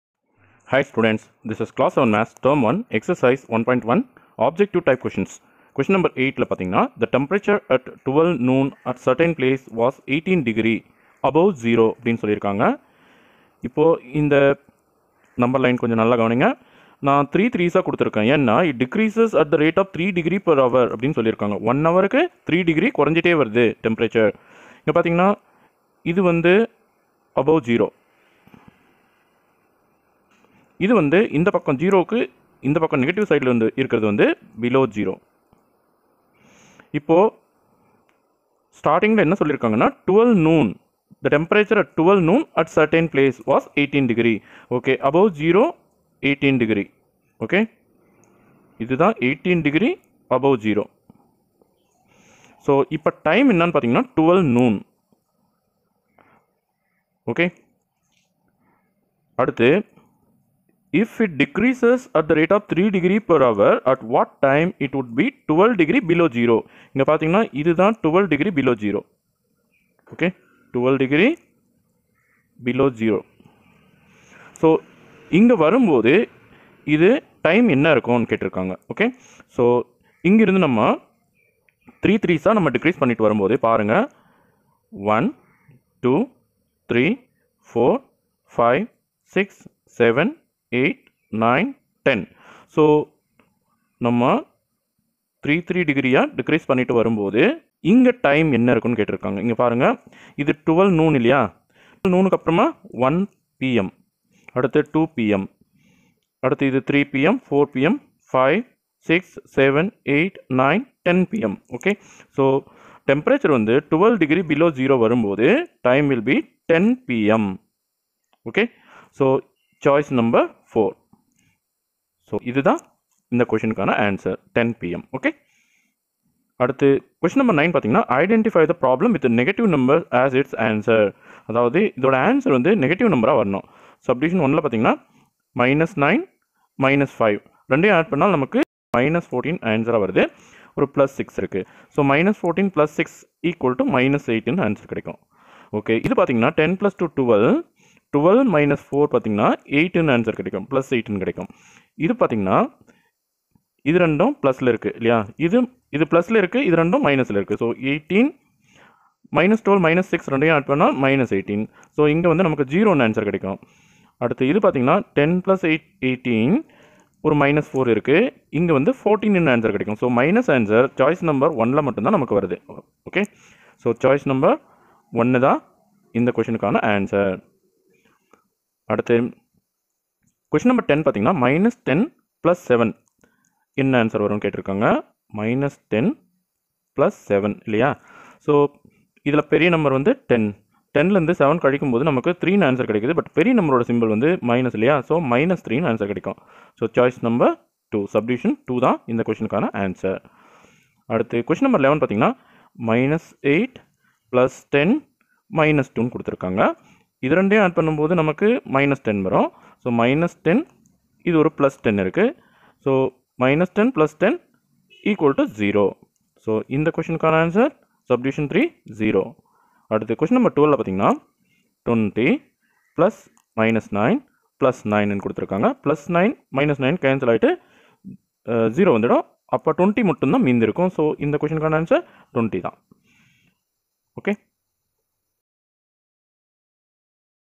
Tn 7th maths term 1 chapter 1 exercise 1.1 qno. 8 to 12
Tn 7TH maths term 1 chapter 1
NUMBER SYSTEM
Exercise 1.1
objective type questions
question 8 to 12explained in Tamil
fill in the blanks
how to workout integers